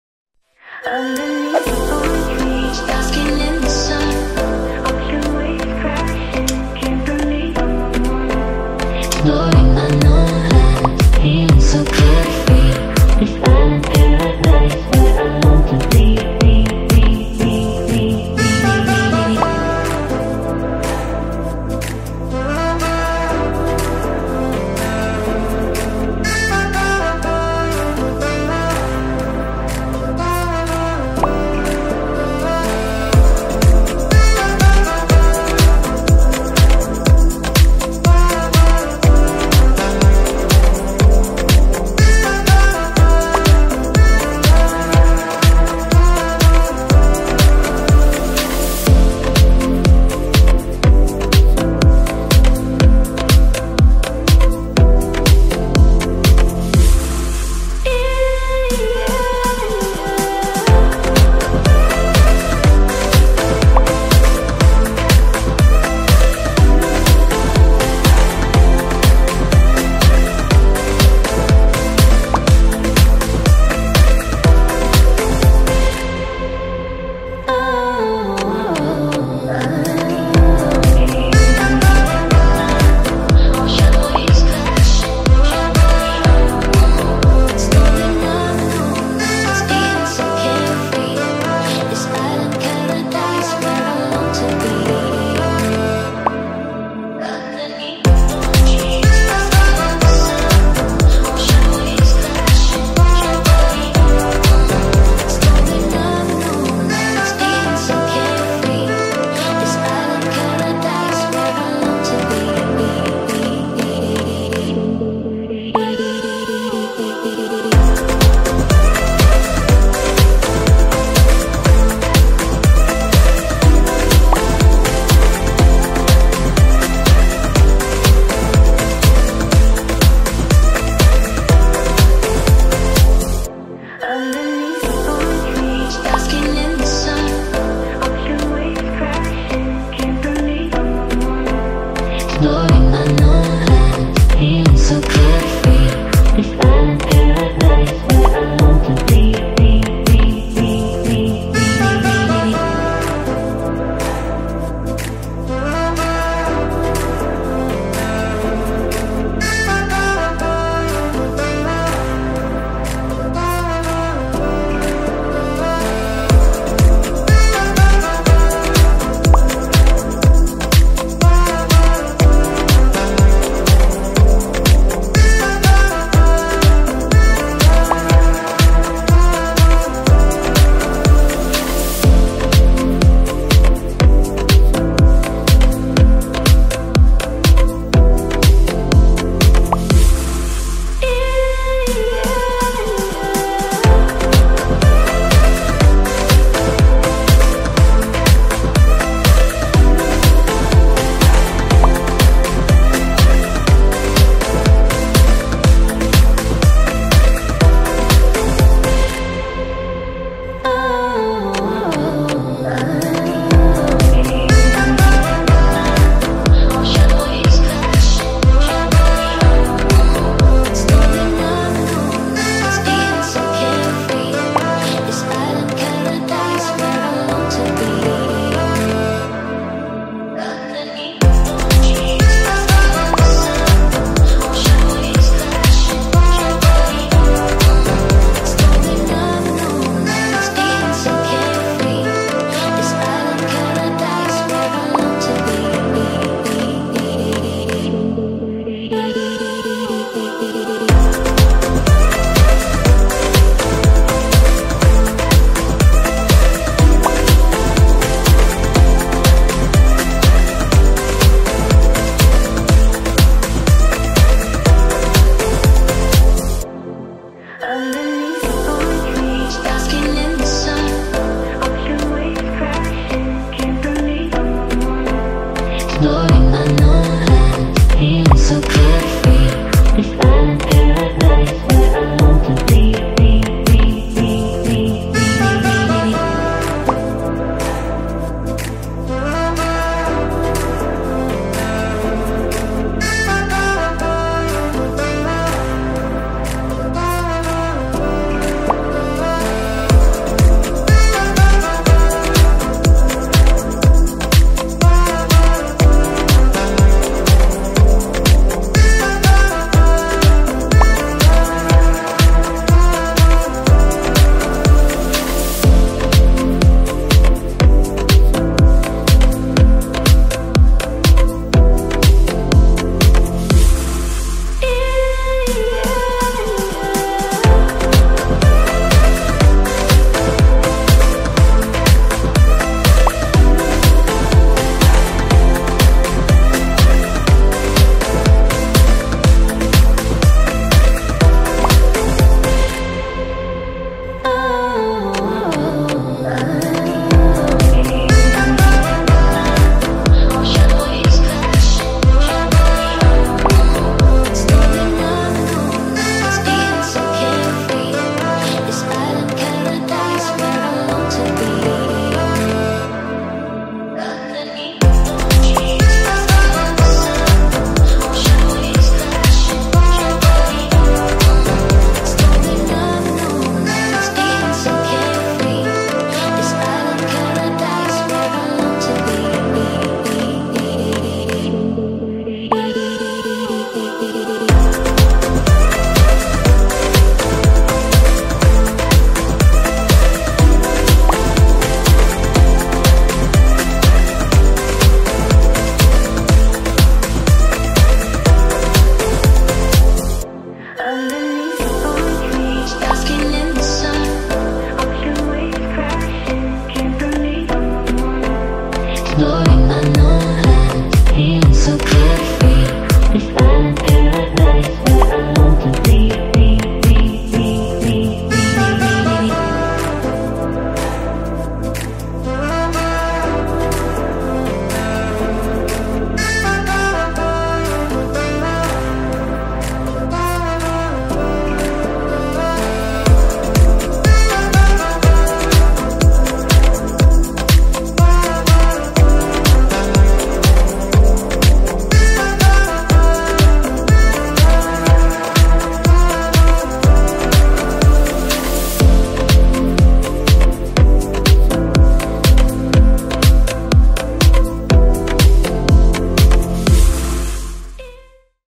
Creative jewellery poster design | Adobe Photoshop tutorial | Yes i'm Graphic Designer
In this video, you will get to know Jewellery Social Media Post design in Adobe Photoshop.